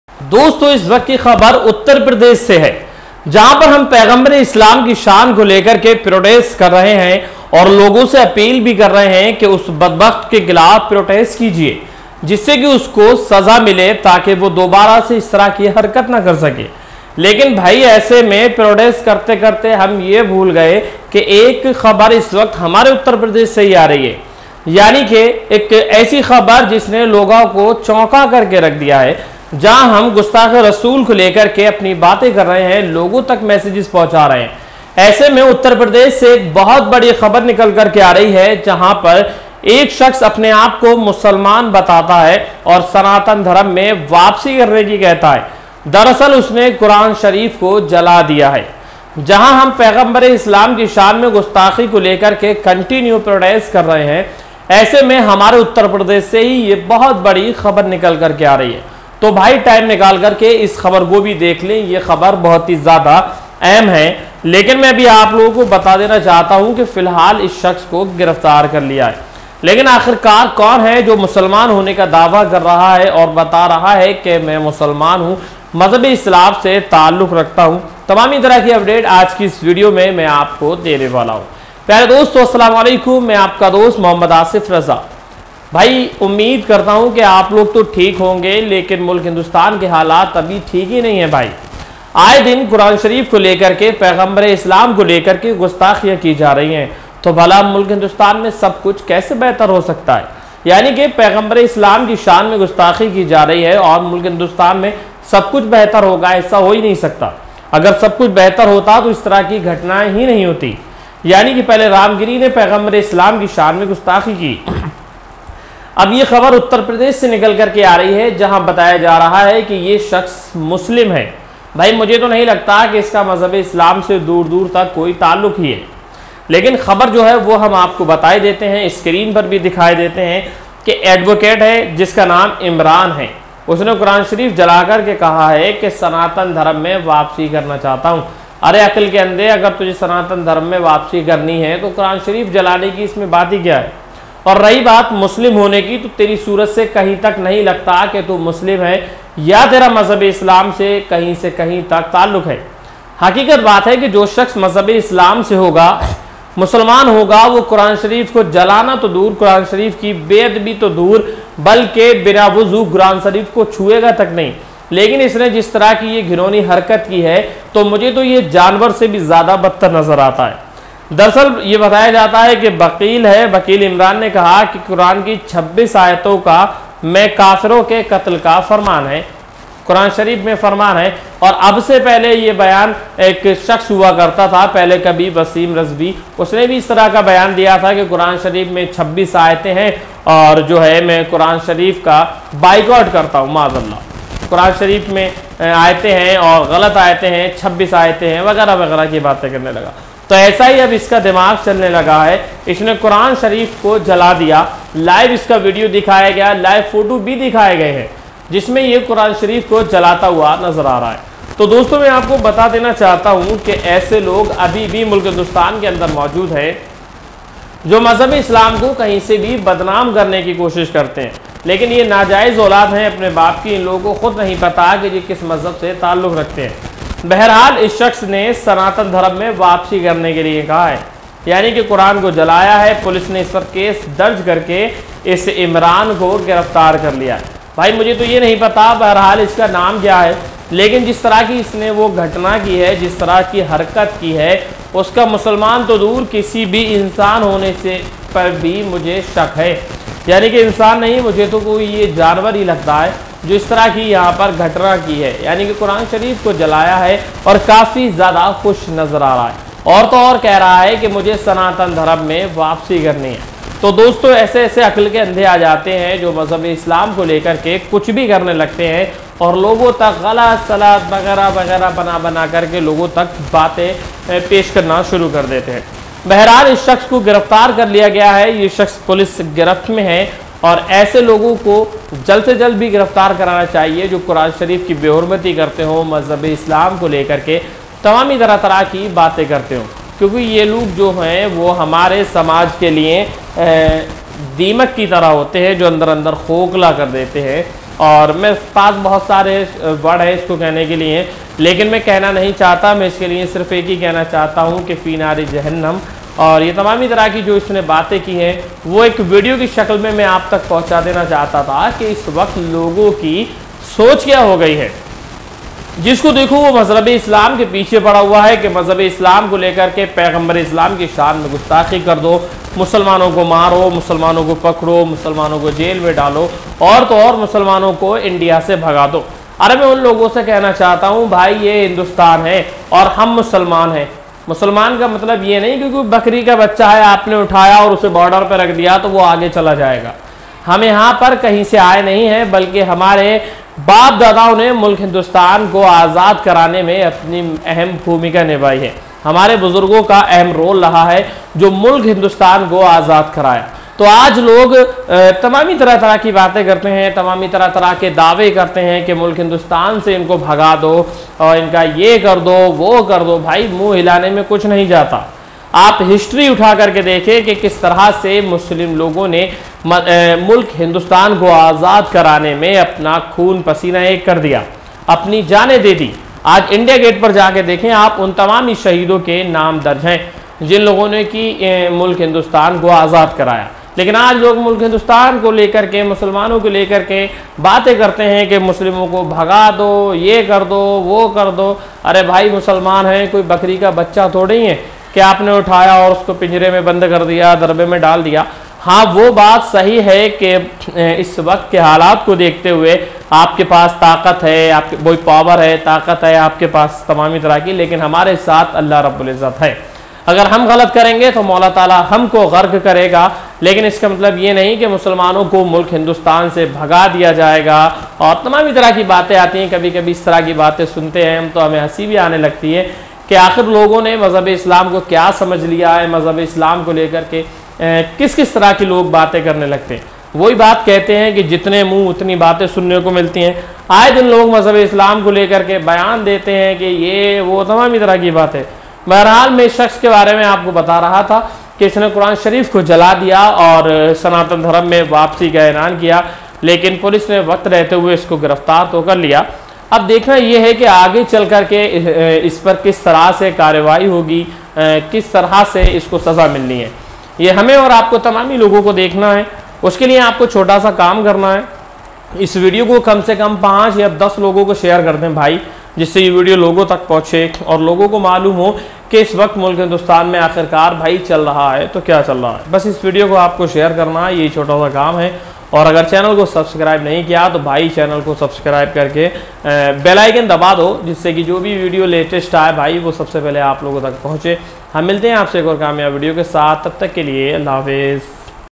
कुरआन की बे अदबी बर्दाश्त नहीं होगी
Aaj ki is video mein ham aapko bataenge ki up ke ek Musalman ne Kuran Sharif ko jala Diya Kuran Sharif ki be hurmati ki,
Kuran Sharif ka ka wo video jiska video social media per kafi teji ke sath viral ho raha hai